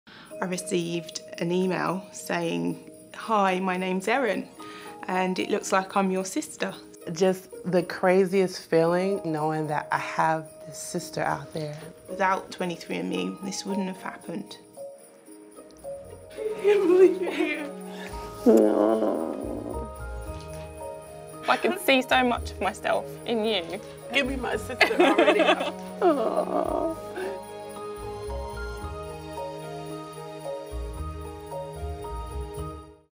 Erin and Edith’s DNA Family Story!
Families meet for the first time with 23andMe! Watch their touching DNA Story, and discover your own by visiting